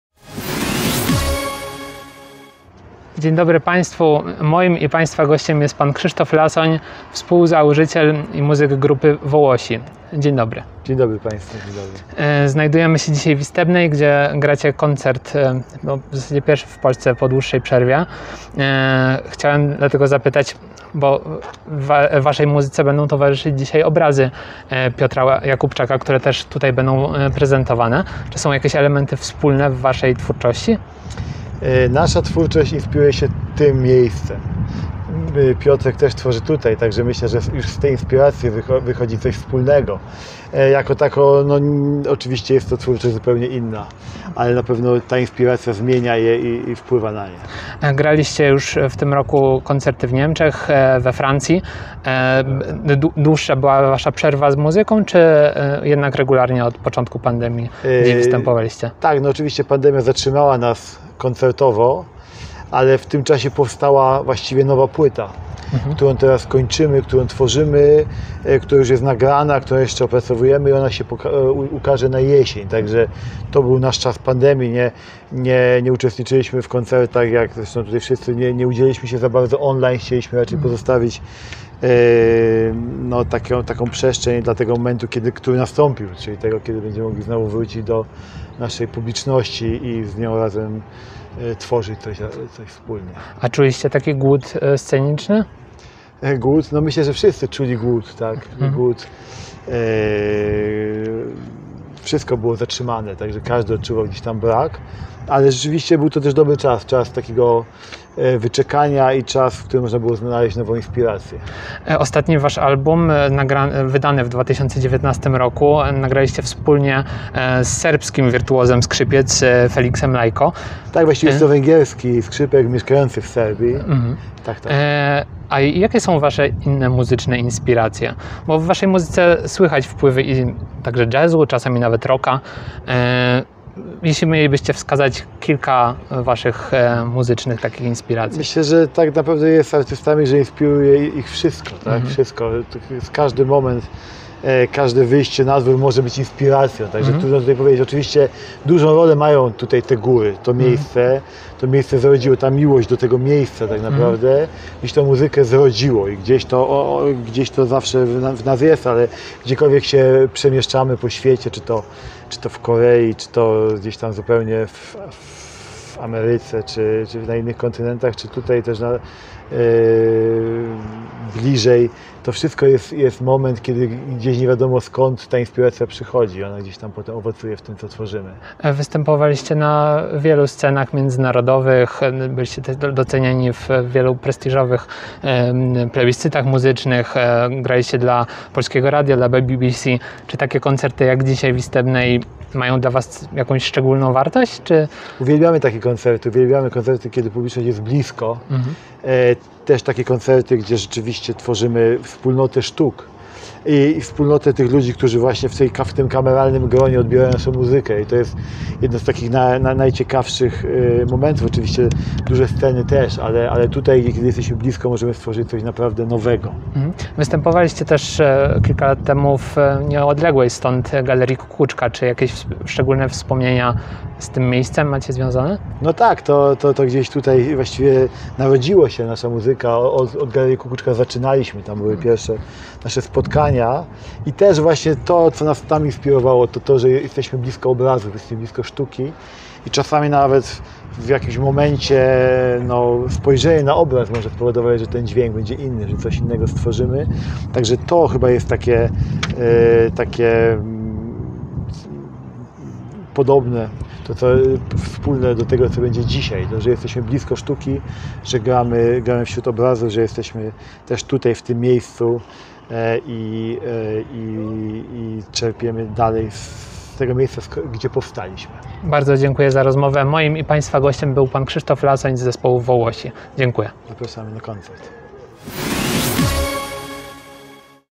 Gość Ox.pl: Krzysztof Lasoń
Przed pierwszymi Beskidzkimi Biesiadami Kulturalnymi, które odbyły się w piątek (16.07) w Istebnej, porozmawialiśmy z Krzysztofem Lasoniem - współzałożycielem grupy Vołosi.